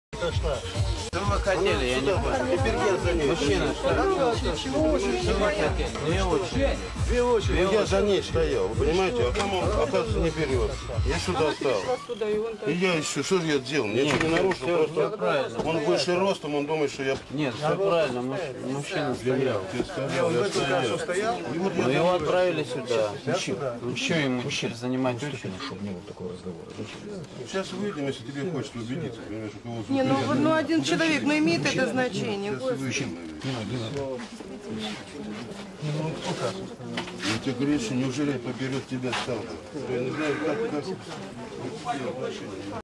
Financial crisis in Russia 1994, 1998 H39
Financial crisis Russia
Financial crisis in Russia 1994, 1998.
The Russian financial crisis (also called "Ruble crisis") hit Russia on 17 August 1998. It was exacerbated by the Asian financial crisis 1997. Many banks were closed down as a result of the crisis.

While financial crisis in Russia
People are queuing to money Exchange office in Moscow,  rubble, the local currency is depreciating very fast, people struggle to be the first in a queue

-Hey man what do you want, there are two  rows of people standing, dont you see?
-I was standing behind  this lady. But the other window is closed so I moved to this one
I obey the rules, but the other man he is taller than me and thus he is saying I ma wrong
Banking worker: of course he is right, he does not need to stay once again. Let him in
-Wellif you want to be loud wait until we get outside , to the street
-One man makes no difference
People get inpatient

Очередь очередной банк народ волнуется кто первый